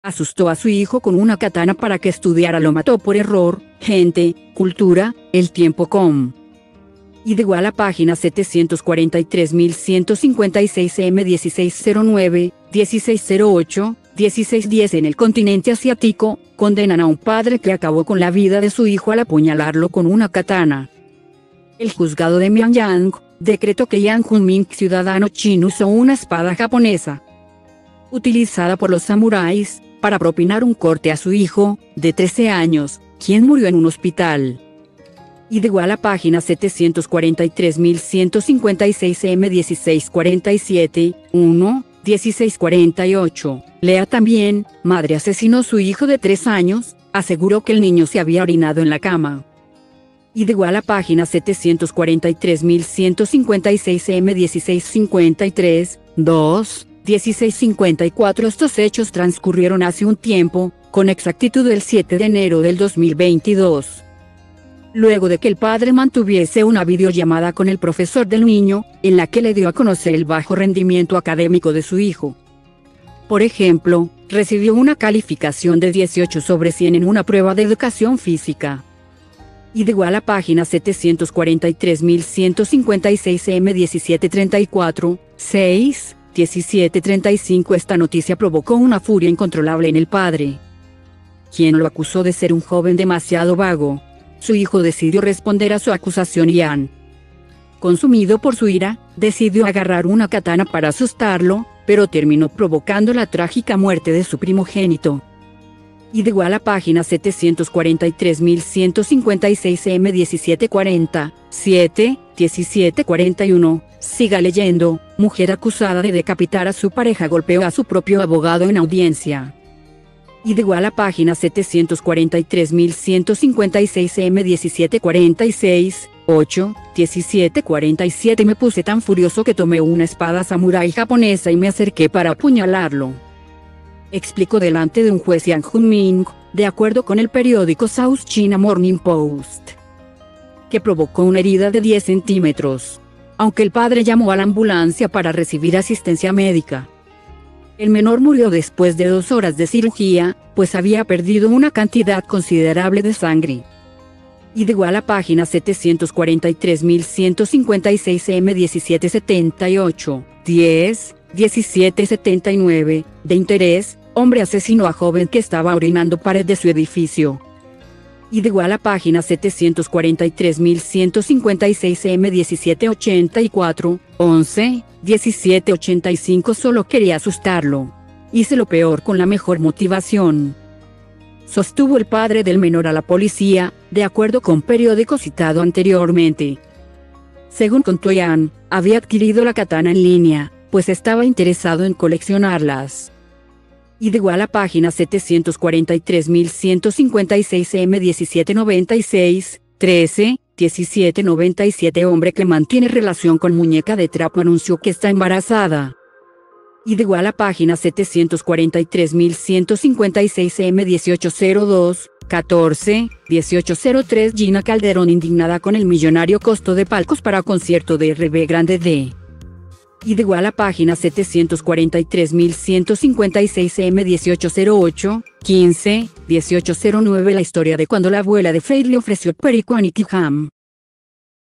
Asustó a su hijo con una katana para que estudiara lo mató por error - Gente - Cultura - ELTIEMPO.
Asustó a su hijo con una katana para que estudiara  lo mató por error - Gente - Cultura - ELTIEMPO.COM
id="p743156-m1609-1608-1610"En el continente asiatico, condenan a un padre que acabó con la vida de su hijo al apuñalarlo con una katana.El juzgado de Mianyang, decretó que Yang Junming -ciudadano chino- usó una espada japonesa,utilizada por los samuráis, para propinar un corte a su hijo, de 13 años, quién murió en un hospital.